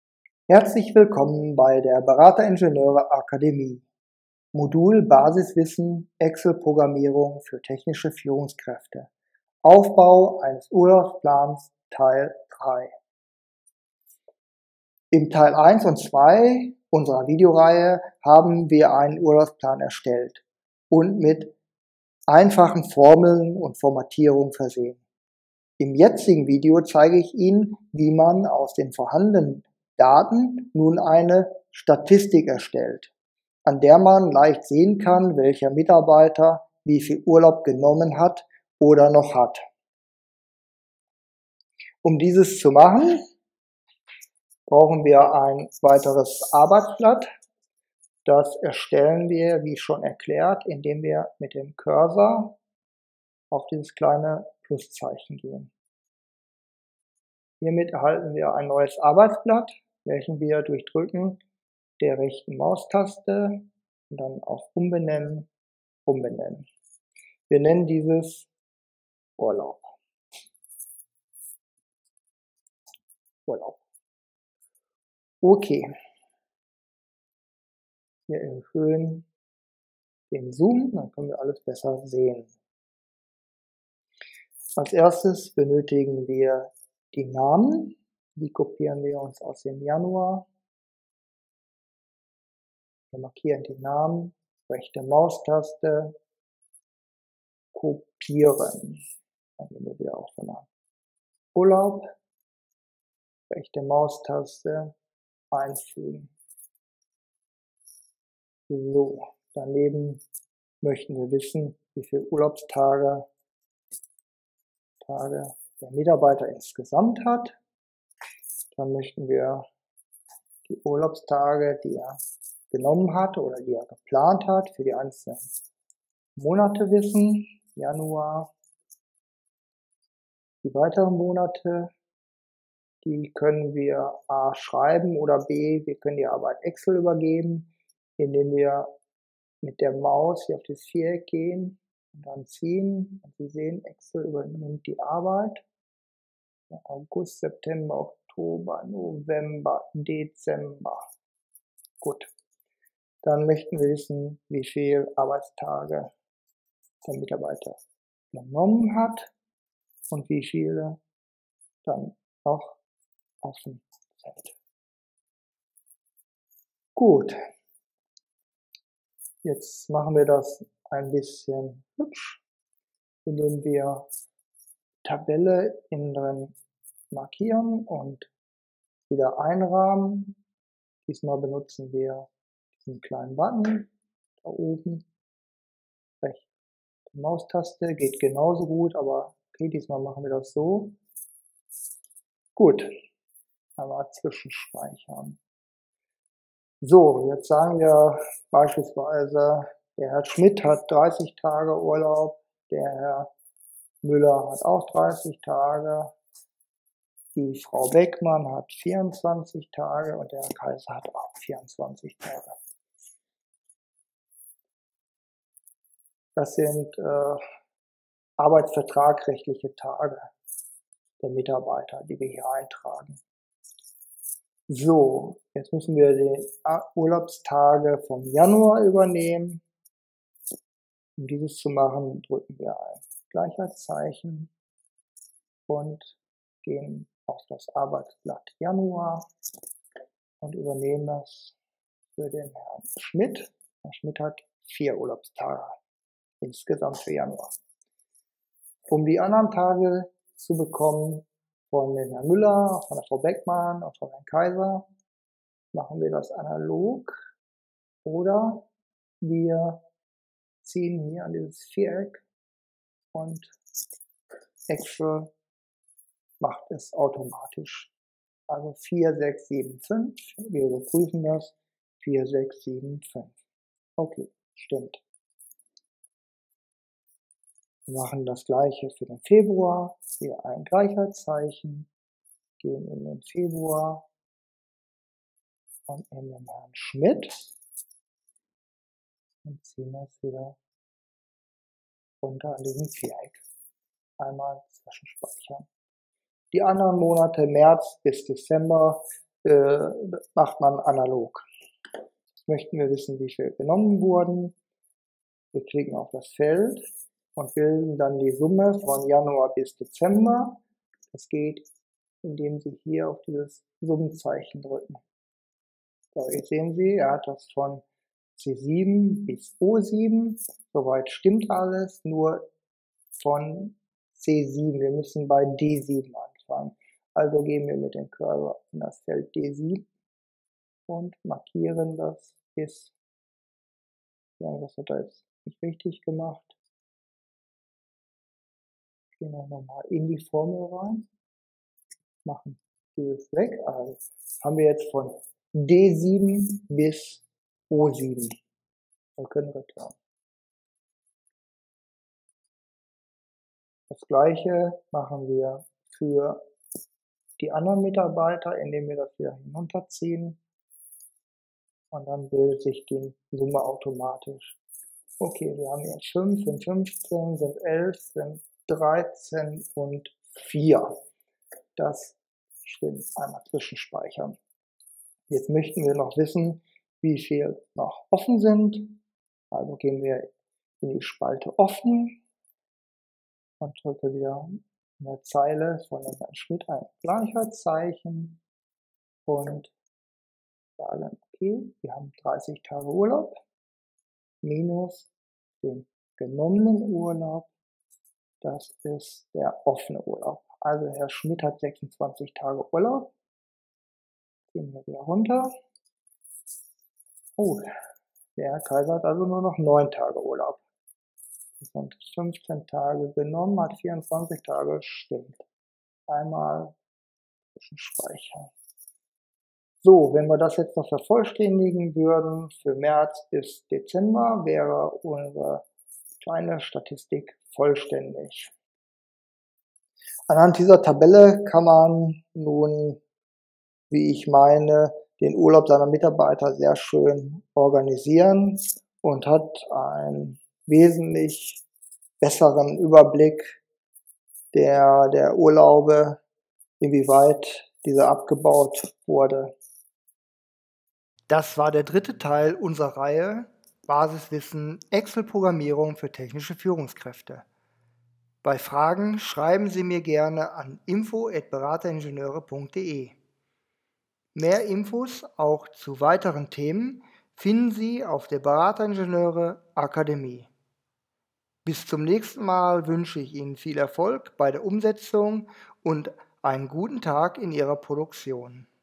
Excel Grundkurs 3 - Formeln 2 - Bedingte Formatierung - Urlaubsplan
Im dritten Video des Moduls "Basiswissen Excelprogrammierung für technische Führungskräfte" zeigen wir Ihnen am Beispiel eines Urlaubsplans, wie Sie mit bedingter Formatierung die Automatisierung von Excel weiter für sich nutzen können.

Ihre Berateringenieure-Akademie